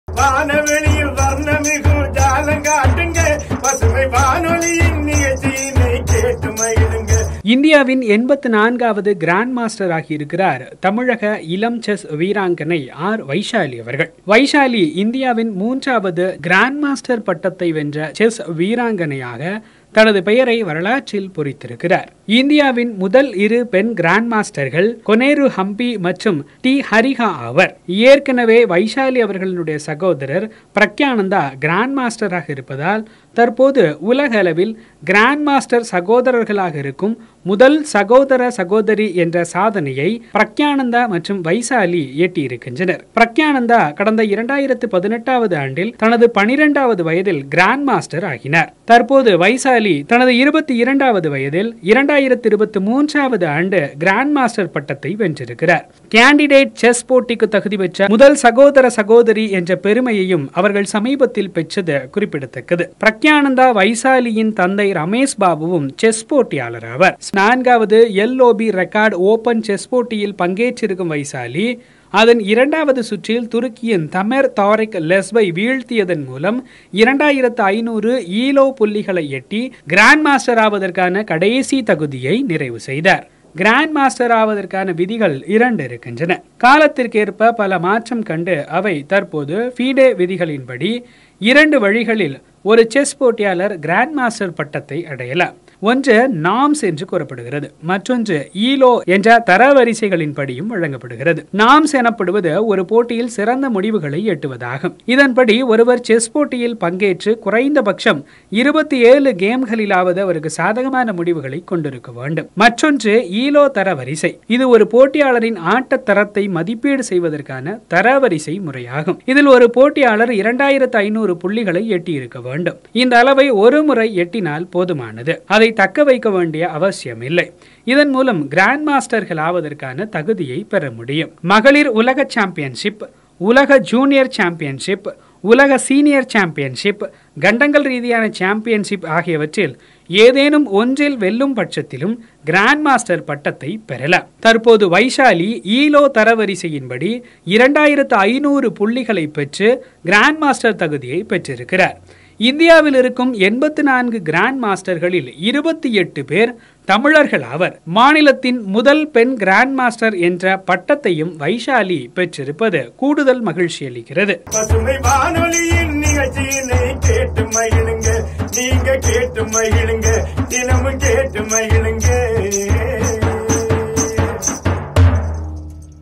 கிராண்ட் மாஸ்டர் வைஷாலி || Pasumai Tv
ஸ்பெயின் நாட்டில் லோப்ரெகட் ஓபன் செஸ் போட்டி நடைபெற்று வருகிறது. இதில் 22 வயதான வைஷாலி 2-வது சுற்றில்துருக்கியை சேர்ந்த ஃபிடே மாஸ்டரான தமேர் தாரிக் செல்ப்ஸை தோற்கடித்தார். இதன் மூலம் வைஷாலி 2,500 எலோ புள்ளிகளை கடந்து கிராண்ட் மாஸ்டர் ஆனார் @chess